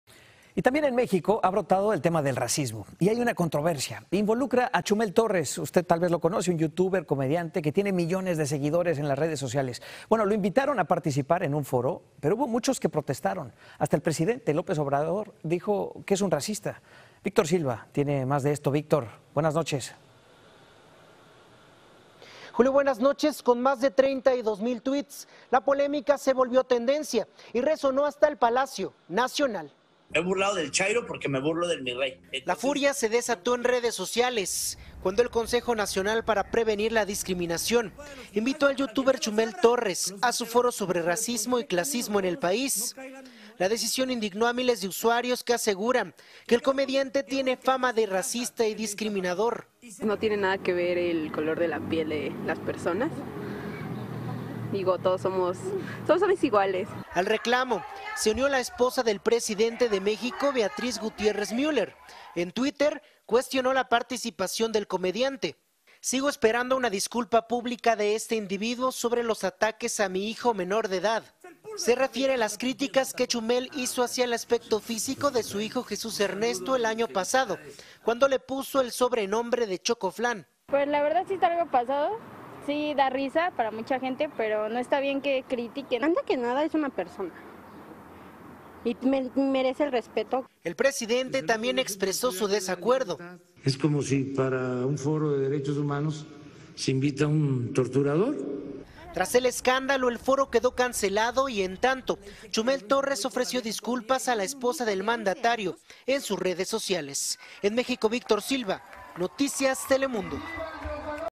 Comediante mexicano con millones de seguidores en las redes sociales es acusado de racista
Video oficial de Noticias Telemundo. La polémica se desató en México cuando el Concejo Nacional para Prevenir la Discriminación invitó a su foro sobre clasismo y racismo a Chumel Torres. Según los usuarios, este comediante tendría fama de discriminador. Víctor Silva amplia la información.